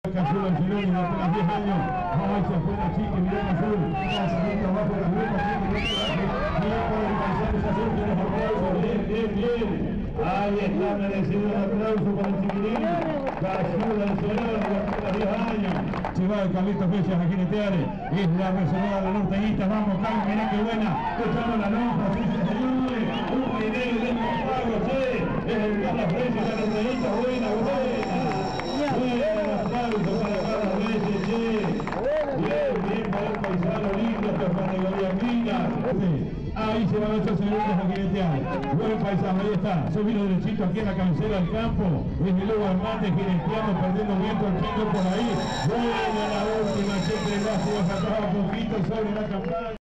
Montas especiales en Rincón de los Sauces, Neuquén (Enero 2012)
FIESTA GAUCHA CON DANIEL SAN LUIS LLEGO OTRA VEZ A LA PATAGONIA PARA TRAERNOS LAS MEJORES JINETEADAS.